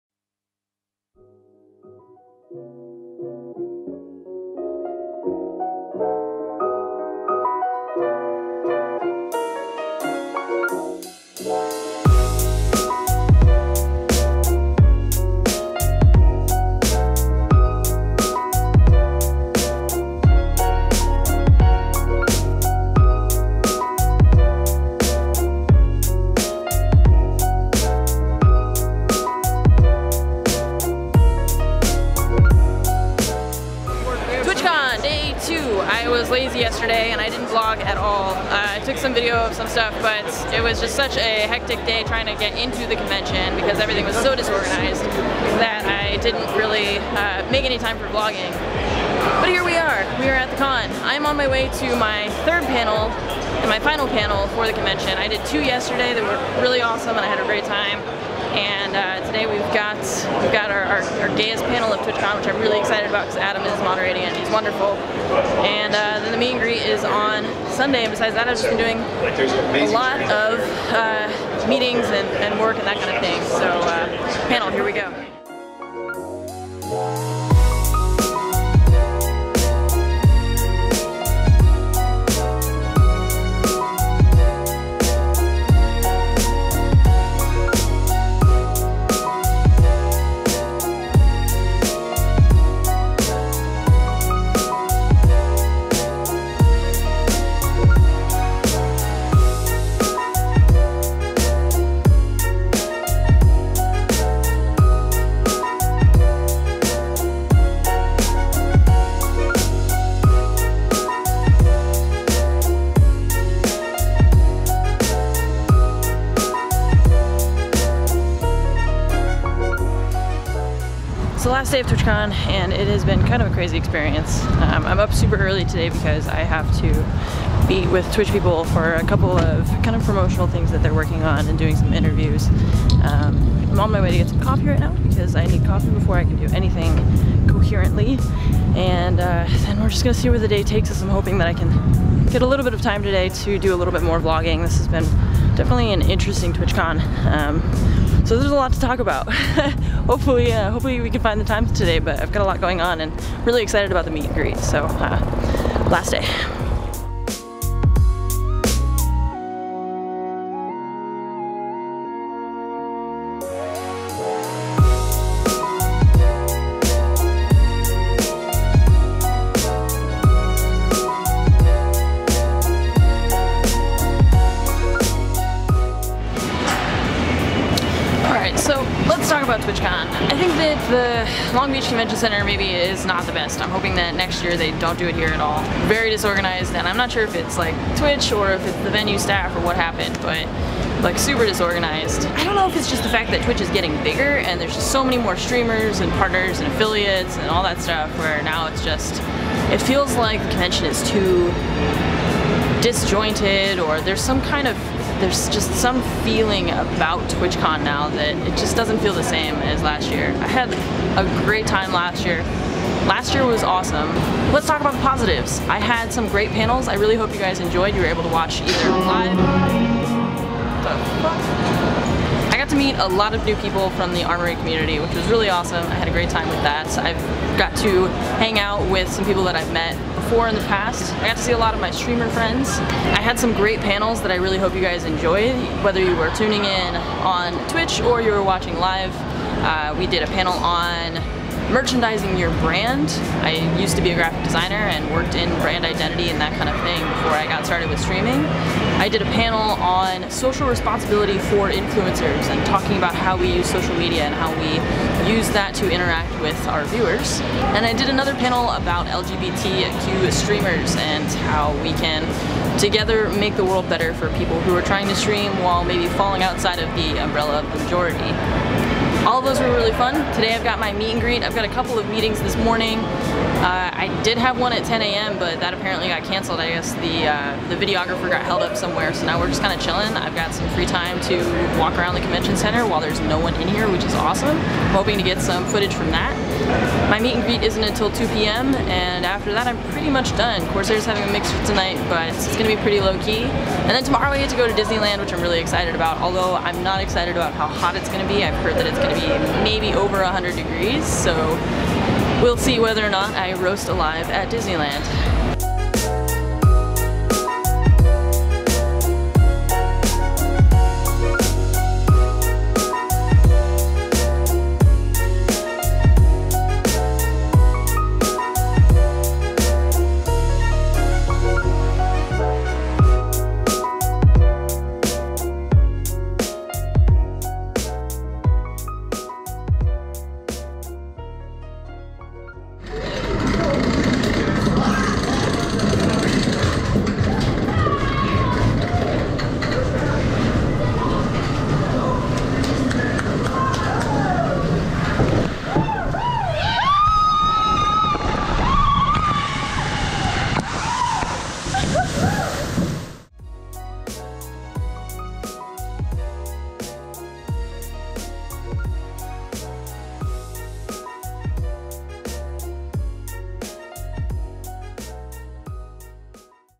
[VLOG] TwitchCon 2017
Kind of a shorter convention vlog, I was honestly just so slammed schedule-wise that I didn't make a ton of time for vlogging.